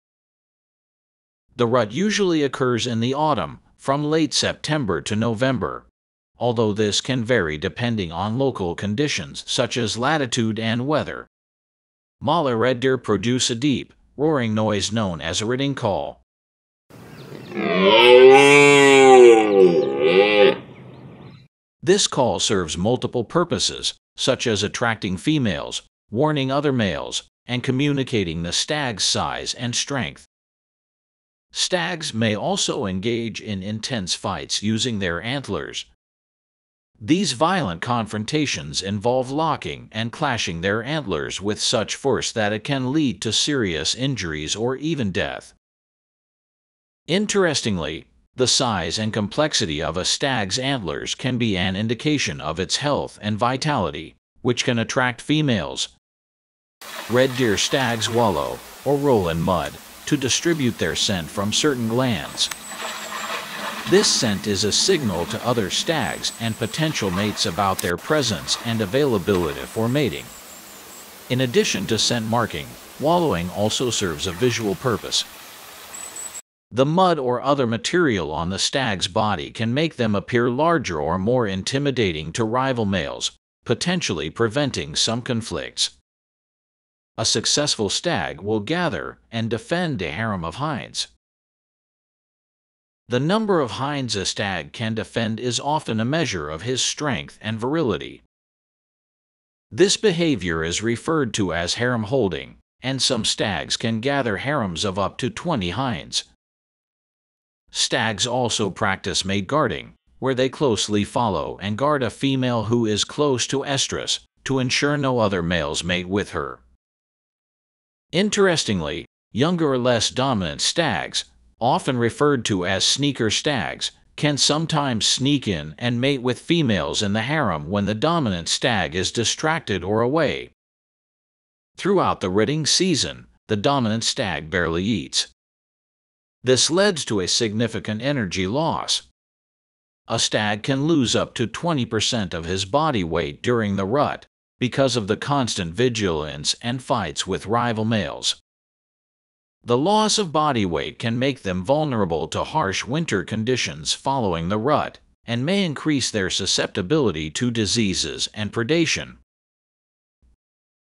Surprising Facts about Red Deers Mating Behaviors | Footage of Red Deers Mating
This video is about the surprising facts about Red deers (Cervus elaphus) mating behaviors, including a real mating footage.

1. Mating Season
The rut usually occurs in the autumn, from late September to November, although this can vary depending on local conditions such as latitude and weather.

2. Rutting Call
Male red deer produce a deep, roaring noise known as a "rutting call." This call serves multiple purposes, such as attracting females, warning other males, and communicating the stag's size and strength.

3. Antler Fights
Stags may also engage in intense fights using their antlers. These violent confrontations involve locking and clashing their antlers with such force that it can lead to serious injuries or even death. Interestingly, the size and complexity of a stag's antlers can be an indication of its health and vitality, which can attract females.

4. Scent Marking and Intimidation
Red deer stags wallow, or roll in mud, to distribute their scent from certain glands. This scent is a signal to other stags and potential mates about their presence and availability for mating. In addition to scent marking, wallowing also serves a visual purpose. The mud or other material on the stag's body can make them appear larger or more intimidating to rival males, potentially preventing some conflicts.

5. Harem Defense
A successful stag will gather and defend a harem of hinds. The number of hinds a stag can defend is often a measure of his strength and virility. This behavior is referred to as harem-holding, and some stags can gather harems of up to 20 hinds. Stags also practice mate guarding, where they closely follow and guard a female who is close to estrus, to ensure no other males mate with her.

6. Dominant Stags and Sneaker Stags
Interestingly, younger or less dominant stags, often referred to as "sneaker" stags, can sometimes sneak in and mate with females in the harem when the dominant stag is distracted or away.

7. Post-rut Exhaustion
Throughout the rutting season, the dominant stag barely eats. This leads to a significant energy loss. A stag can lose up to 20% of his body weight during the rut, because of the constant vigilance and fights with rival males. The loss of body weight can make them vulnerable to harsh winter conditions following the rut, and may increase their susceptibility to diseases and predation.

Media Credits : Film Footage courtesy of Shutterstock, Inc.,
(author and title)

Philippe Clement - Red deer (Cervus elaphus) stag mounting doe / female in heat in forest during the rut in autumn / fall
Grisha Bruev - Male European Red Deer Or Cervus Elaphus Have A Roar During Rut. Red Deer Inhabits Most Of Europe, Caucasus Mountains Region, Parts Of Asia. Male Deer Roaring In Autumn Forest.
Stephan Morris - Red Deer Stag. Head shot with below.
shaftinaction - A pair of Red Deer stags battle it out in a shaded woodland.
Chamois huntress - red stag, cervus elaphus, in the rutting season on the mountains in a mud bath at a autumn evening
JaklZdenek - Red deer in the natural environment, wild animal, close up, Cervus elaphus
Philippe Clement - Red deer (Cervus elaphus) stag with branch in huge antlers bellowing in forest during the rut in autumn
MM.Wildlifephotos - Red deer male lie on the meadow with females, mating season, autumn, germany, (cervus elaphus)
MM.Wildlifephotos - Red deer male is calling on the meadow, mating season, autumn, germany, (cervus elaphus)
Philippe Clement - Rival male in the background watching red deer (Cervus elaphus) stag bellowing among hinds in grassland at forest's edge during the rut in autumn / fall
Edwin Godinho - The red deer rut begins in September and lasts until around early November. During this time, competing males, pumped full of testosterone, including bellowing.
LukasPich - Red deer (Cervus elaphus) resting in forest clearing
David Fitzell - Red Deer Glen Etive, Scotland